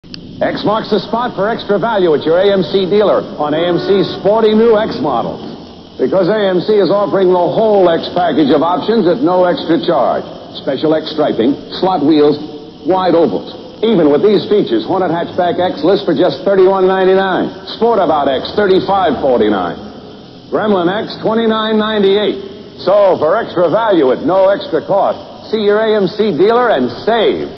1976 AMC X-Model Commercial
Commercial of 76 AMC Gremlin-X,Hornet-X and Sportabout-X